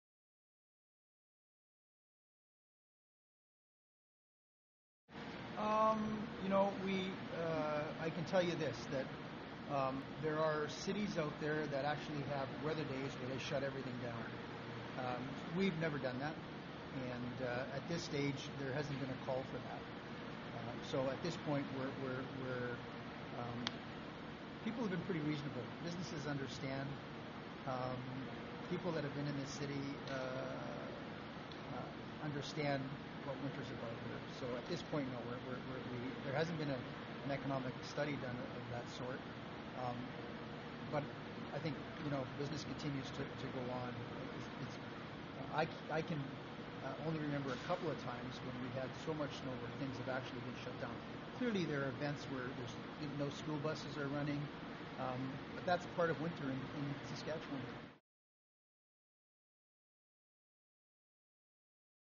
Mayor Pat Fiacco speaks on snow removal and its economic impact
Mayor Fiacco held his annual winter road maintenance kickoff on Nov. 16, 2010. He spoke to reporters about snow removal and its impact on the City of Regina's economy.